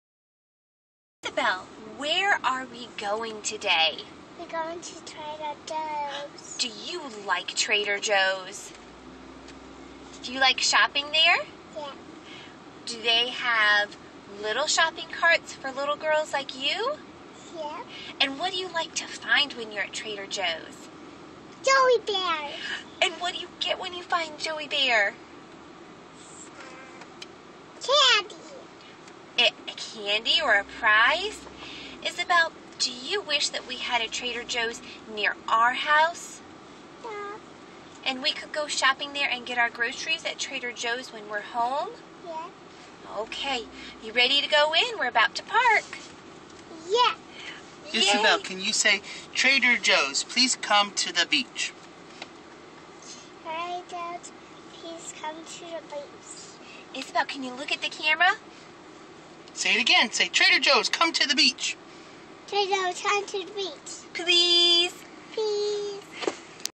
Trader Joe's Please Come To Myrtle Beach
A plea for Trader Joe's to come to Myrtle Beach by my daughter.  She loves visiting the one in Northern Atlanta so much that she wants one here in Myrtle Beach now.